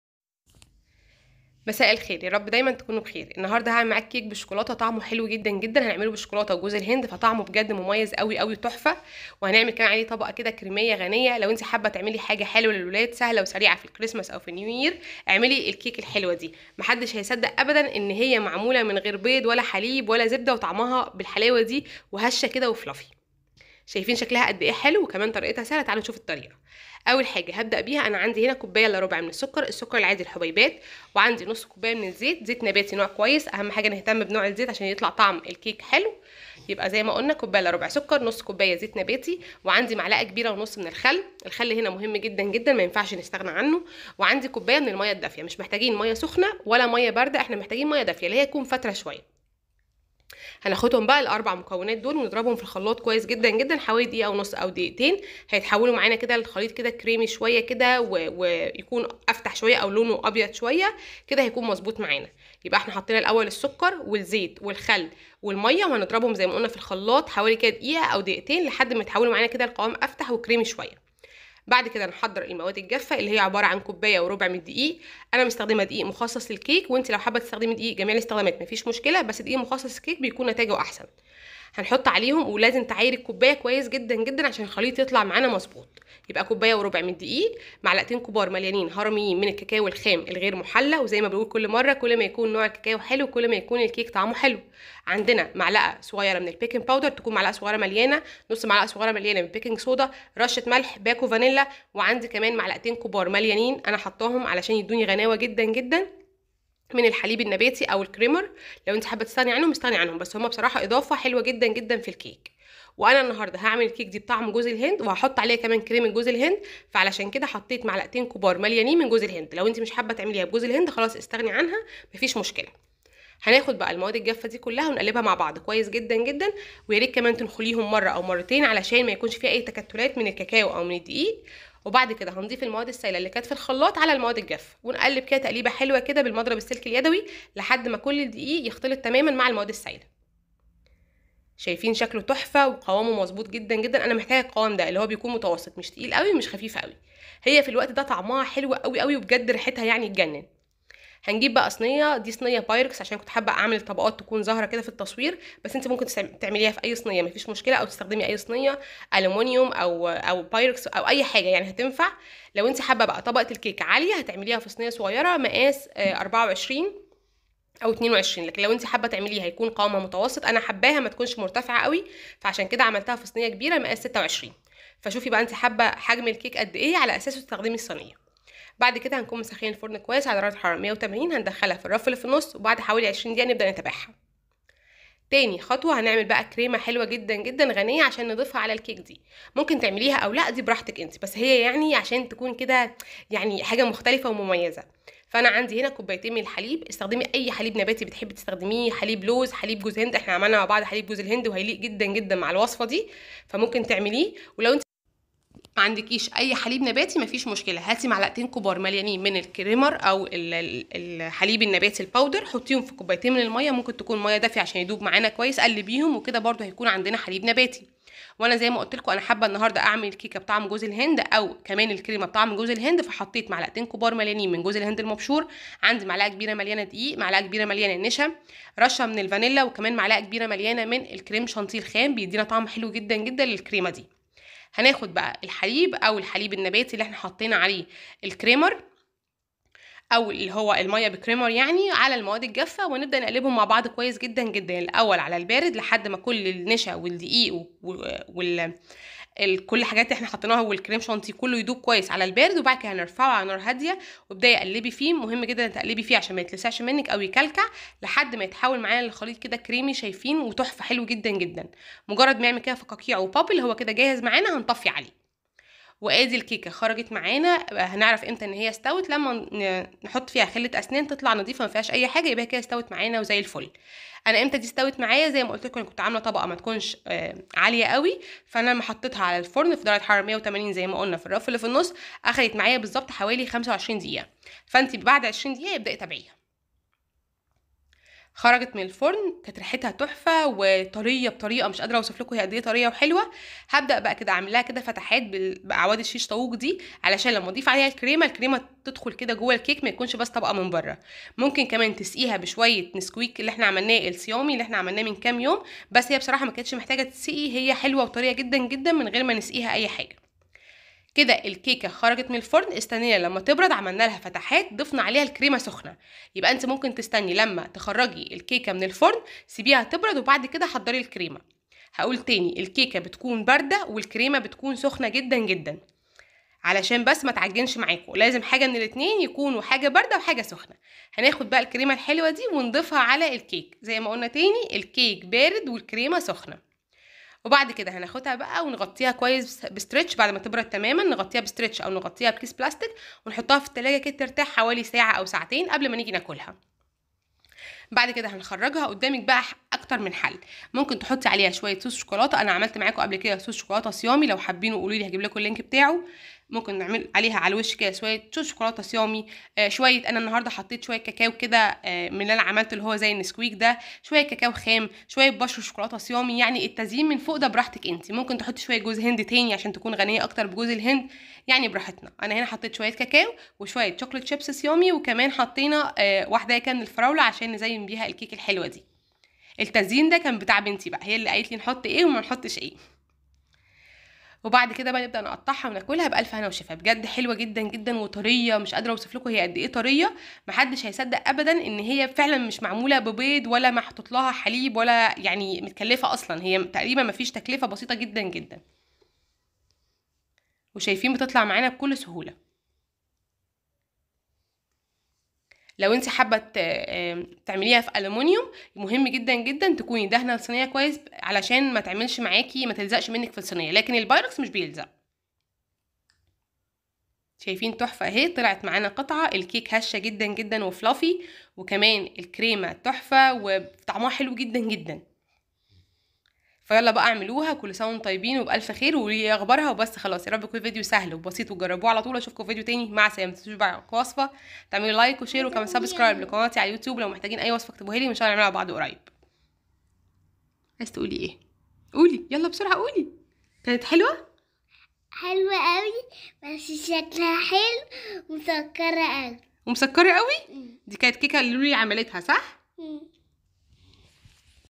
بدون بيض وحليب هنعمل كيك شوكولاتة هش جدااا وغني وتحفة بجد لازم تجربوها
المقادير:
كوب وربع دقيق كيك او دقيق عادي المهم نعاير الكوب كويس الكوب بتكون ١٥٠ جرام
رشه ملح ، رشه فانيلا ، م ص مليانه بيكنج بودر
نص م ص مليانه بيكنج صودا
٢ م ك مليانه جوز هند اختياري
٢ ك م مليانه كاكاو خام نوع كويس
٢ م ك مليانه كريمر او اي حليب نباتي
نص كوب زيت  ، كوب الا ربع سكر
كوب ماء دافي  ، م ك ونص خل
الصوص الكريمي  :
٢ كوب حليب نباتي او كريمر وضحت الطريقه فالفيديو
رشه فانيلا  ، م ك مليانه دقيق ، م ك مليانه نشا
٢ م ك جوز هند  ، ولو في اسنس جوز هند نحط نص م ص
او من غير جوز هند خالص لو مش عايزين
 الوصفه بجوز الهند ، م ك كريم شانتي خام
سكر حسب الرغبة حوالي ٢ م ك
للتزيين  : صوص شوكولاتة ، كاكاو خام ، قطع شوكولاتة  ، نسكويك حسب الرغبة وبس جربوها تحفه 🥰